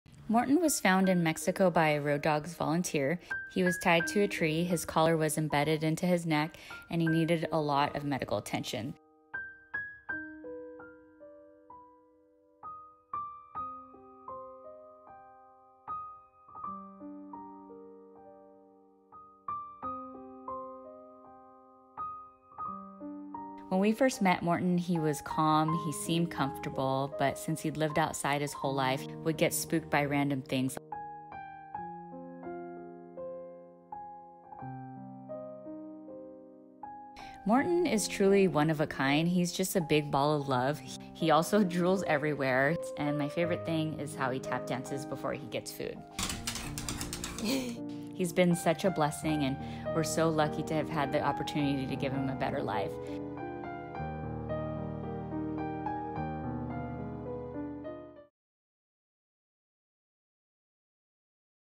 Road Dogs' Morton Cadbury Comes Back to Life!
Morton Cadbury was found as a stray in Mexico, tied to a tree with a collar embedded in his neck. Road Dogs rescued him, brought him to the US, and got him the medical care he needed. With lots of love from his foster and adoptive families, Morton now lives a beautiful, loved life!